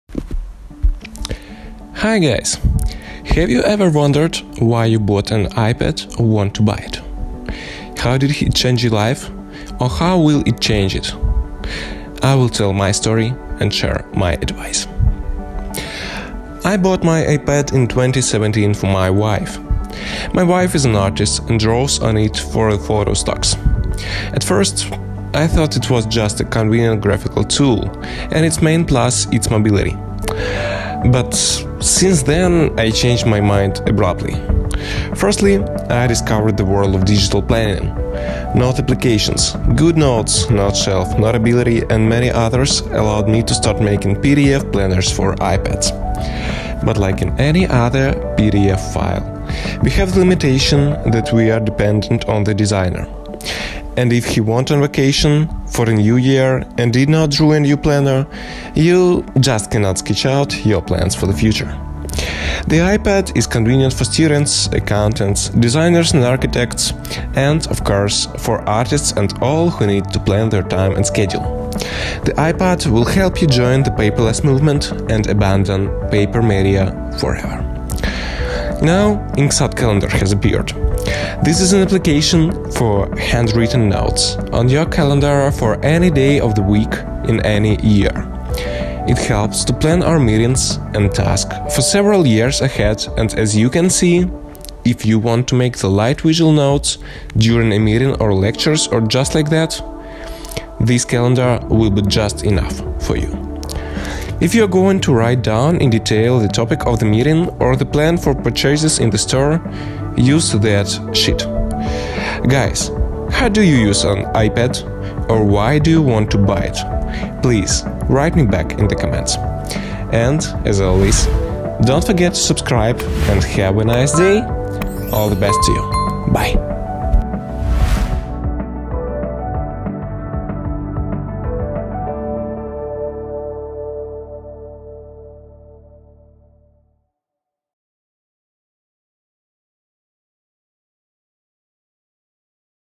Why do you need iPad for digital planning and drawing? Сhange your PDF planner to digital calendar
Digital Calendar for iPad and Android devices for handwriting notes

Hi guys. Have you ever wondered why you bought an iPad or want to buy it? How did he change your life, or how will he change it? I will tell you my story and share my advice.
I bought my iPad in 2017 for my wife. My wife is an artist and draws on it for photo stocks. At first, I thought it was just a convenient graphical tool and its main plus its mobility. But since then I changed my mind abruptly) Firstly, I discovered the world of digital planning. Note application - goodnotes, noteshelf, notability and many others - allowed me to start making PDF planners for iPads. But like in any other PDF file, we have the limitation that we are dependent on the designer) and if he went on vacation for the new year and did not draw a new planner, you cannot sketch out your plans for the future). The iPad is convenient for students, accountants, designers and architects, and of course for artists and all who need to plan their time or schedule. The iPad will help you join the paperless movement and abandon paper media forever.
 Now Inksot Calendar has appeared - this is an application for handwritten notes on your calendar for any day of the week in any year) it helps to plan our meetings and tasks for several years ahead and as you can see - if you want to make light visual notes during a meeting or lectures or just like that - this calendar will be enough for you)
If you're going to write down in detail the topic of the meeting or the plan of purchases in the store - use that du sheet.
Guys, how do you use your iPad or why do you want to buy it - write in the comment.

This group belongs to designer Andre Hryshchenko